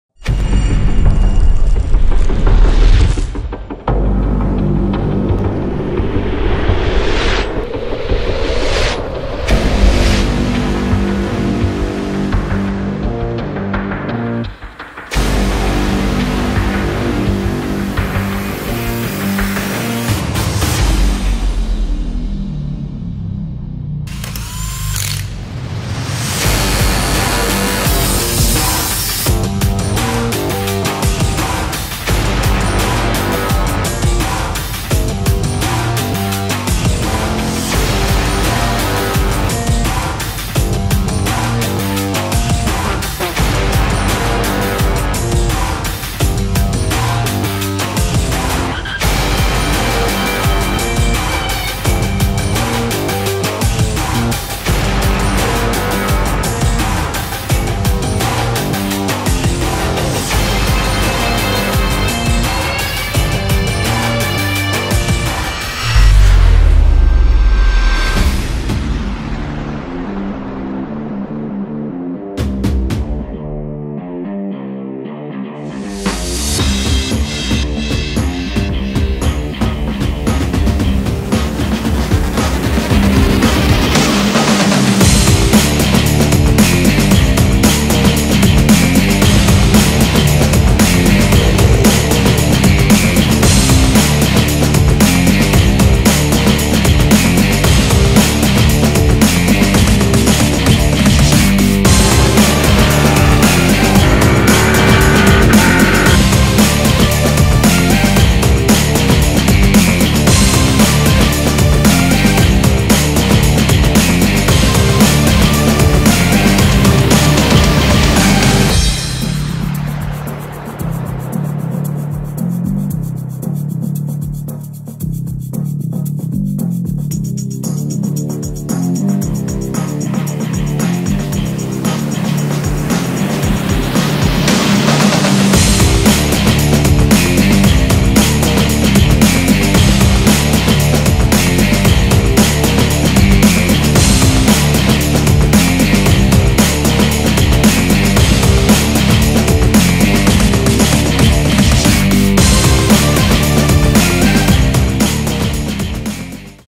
BKR bedrijfsfilm
Is het de nieuwe fast & furious? Nee. Het is de nieuwe bedrijfsfilm voor Bas Koeten Racing.
Voor de bedrijfsfilm van Bas Koeten Racing hebben we een aantal dagen meegelopen op verschillende locaties, zowel in de werkplaats als in de praktijk op het Circuit in Zandvoort.